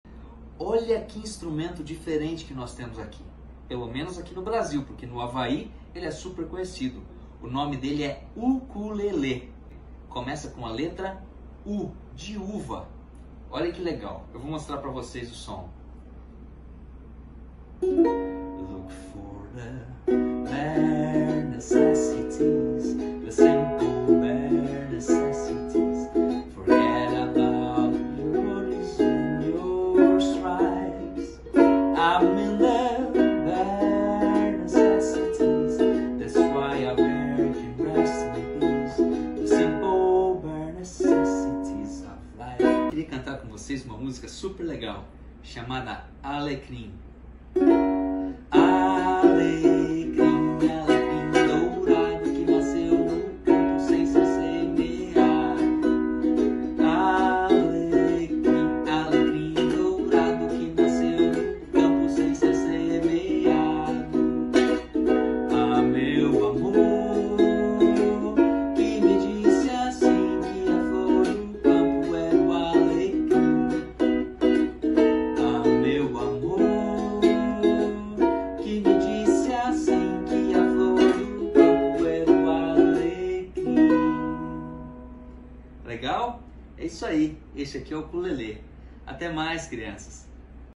Jardim I 22/06
Aprendendo mais sobre instrumentos musicais